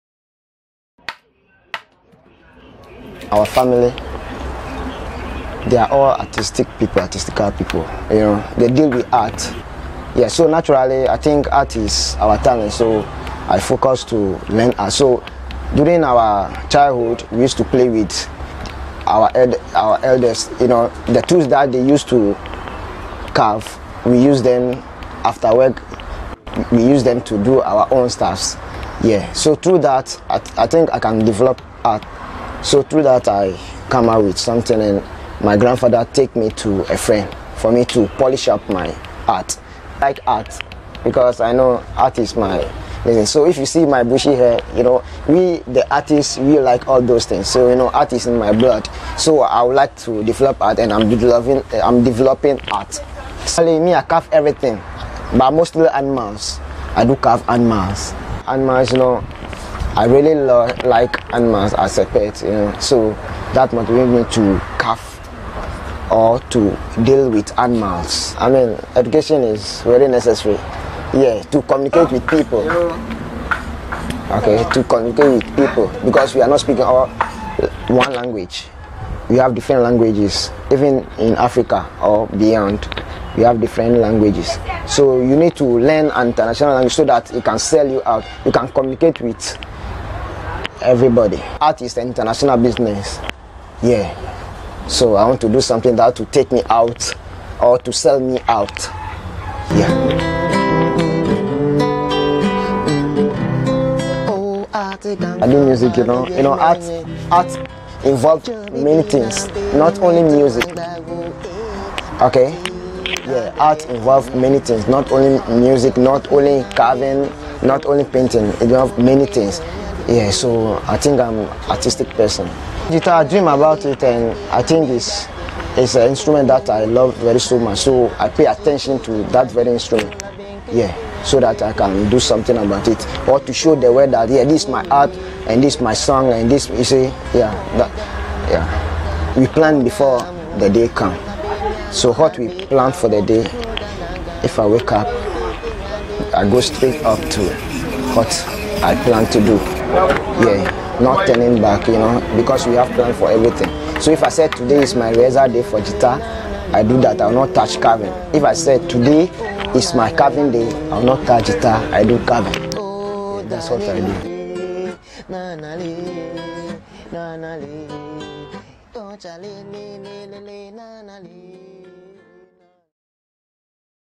Artistic carver
Theophilus Kekrebise is an SHS dropout who now carves for a living. He lives at shiashie in the Greater Accra region of Ghana. He is also passionate about music and plays the guitar as a hobby. Currently he has plans of furthering his education after making enough money from carving

Produced by
Akpene Avor